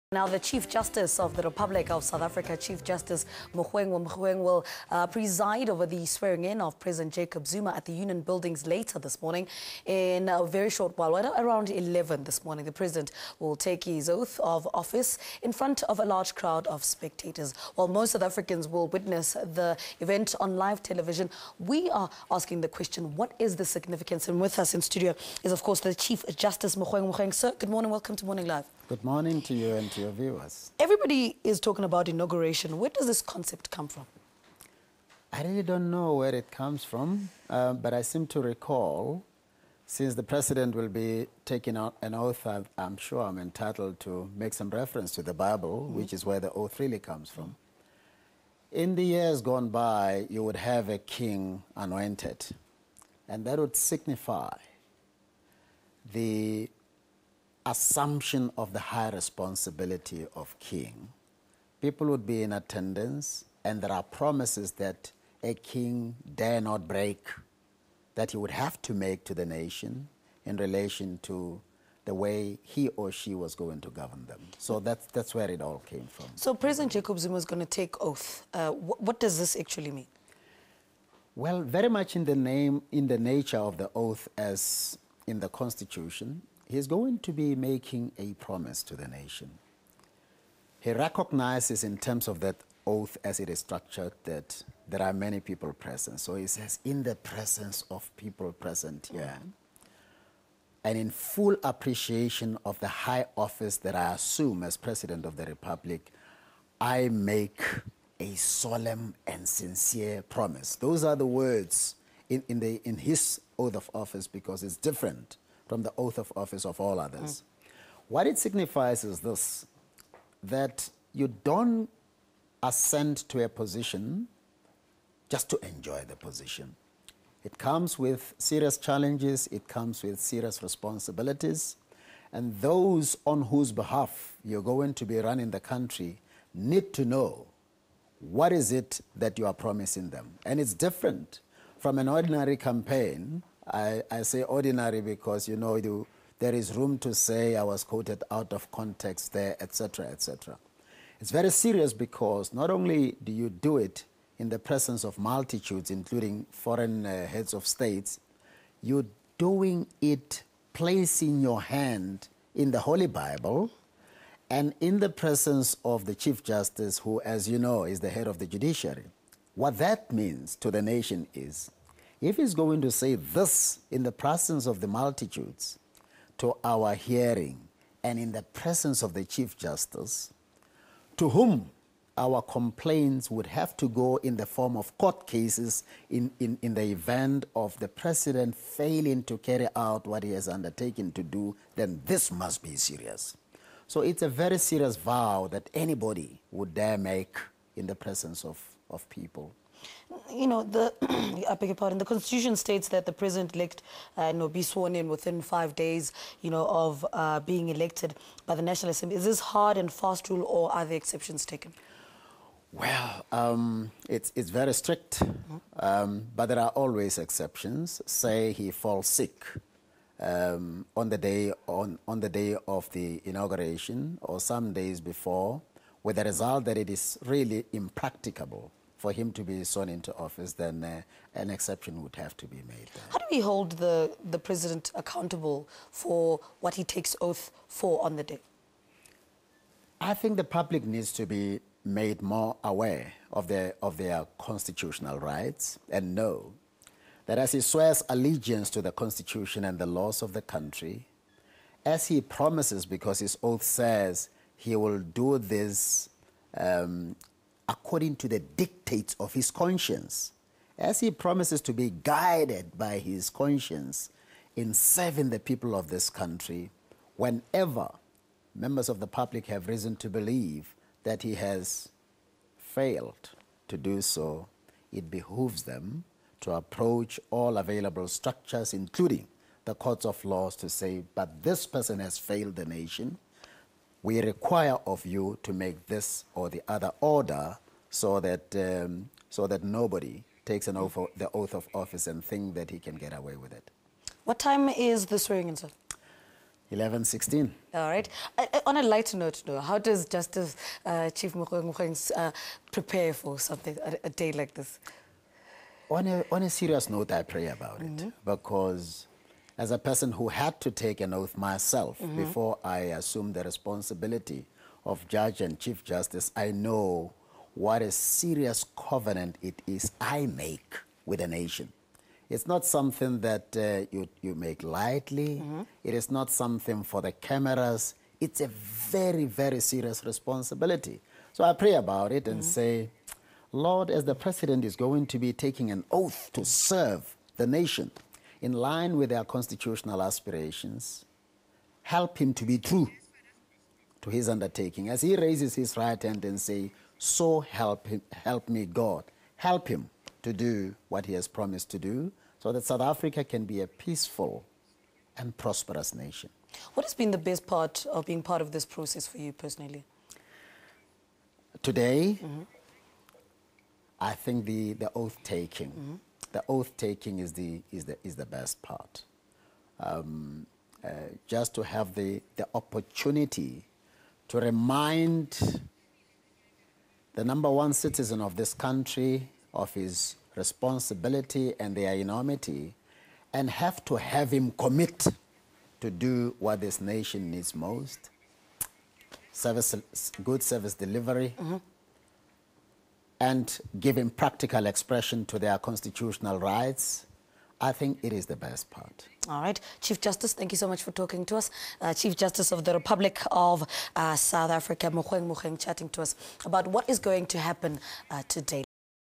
Mogoeng will preside over the swearing in of president Zuma
The chief justice of the republic of South Africa, chief justice Mogoeng Mogoeng will preside over the swearing in of president Zuma here at the union buildings this morning. In a short while the president will take his oath of office in front of a large crowd of spectators, while most South Africans will witness this event on live television.